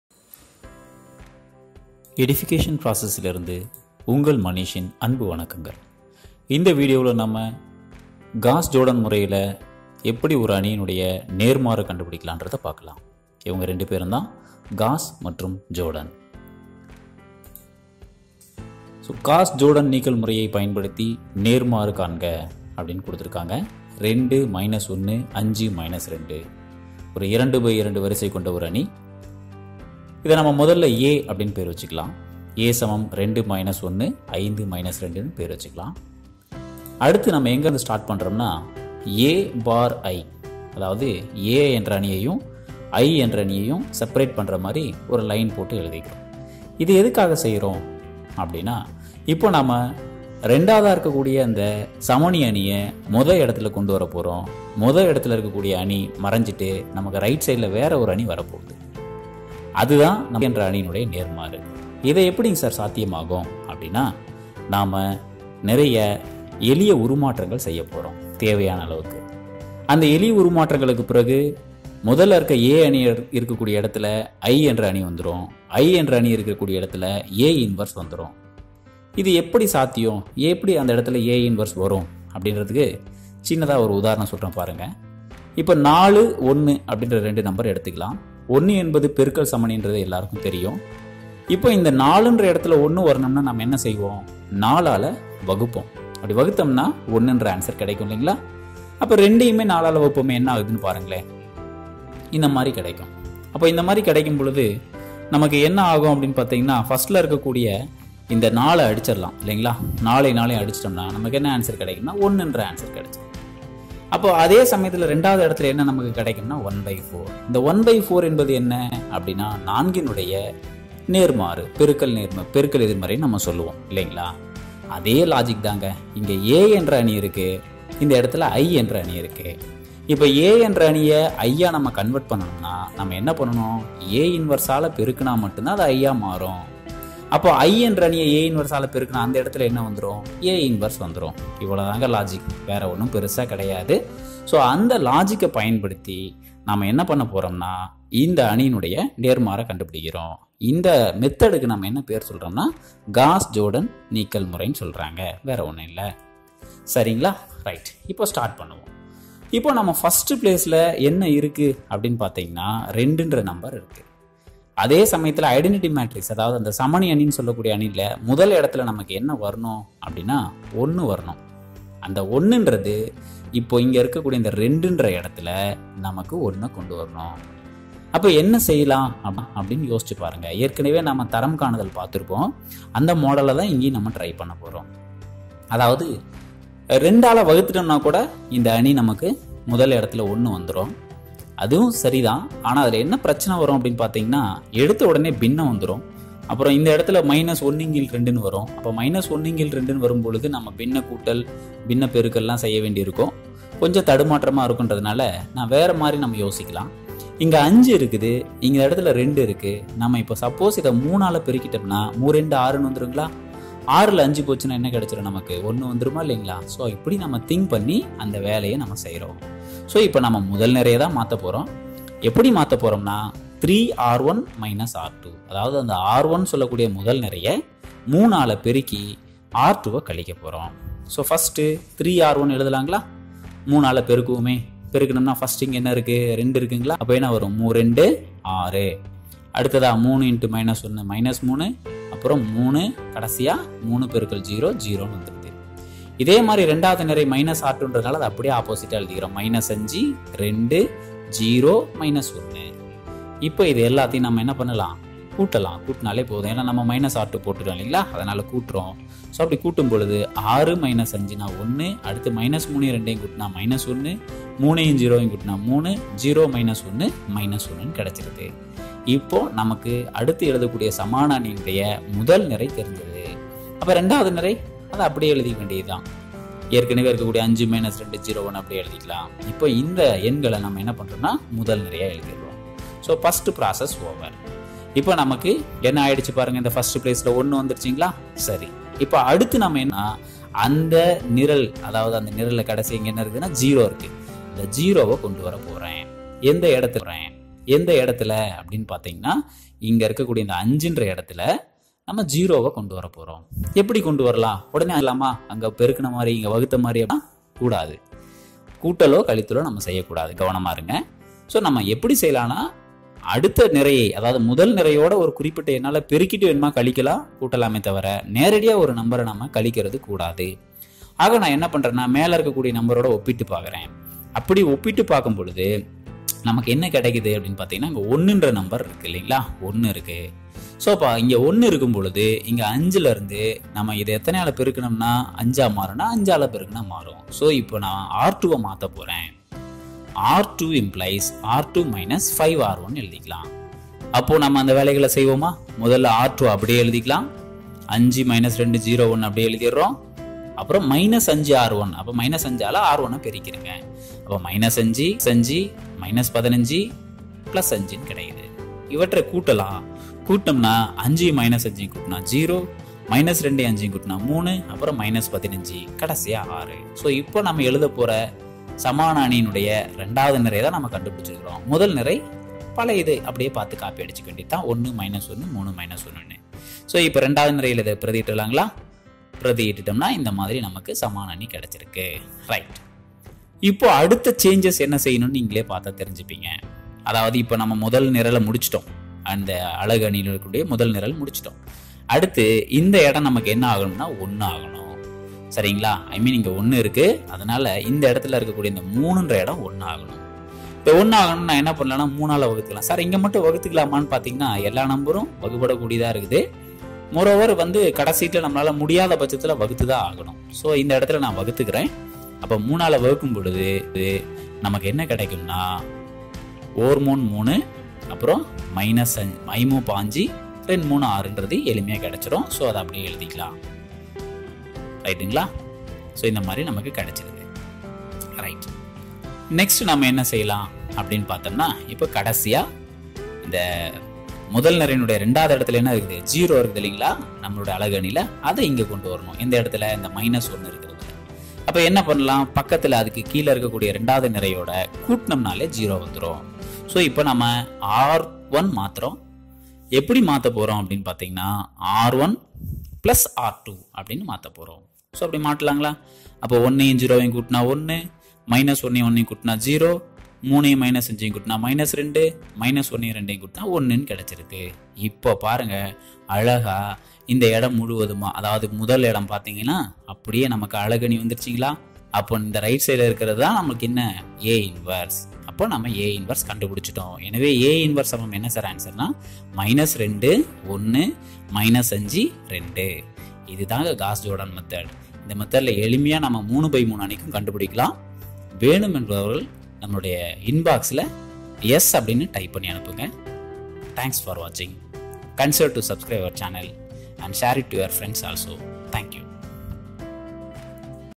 காஸ் ஜோர்டன் முறை | நேர்மாறு காணல் | CLASS 12 | EXERCISE 1.2 | Q.NO 3
காஸ் ஜோர்டான் முறை | நேர்மாறு காணல் | CLASS 12 | EXERCISE 1.2 | Q.NO 3
Understand the concept behind Gauss Jordan method
Find the inverse of a matrix using Gauss Jordan Method
Step by step explanation with eyecatching animations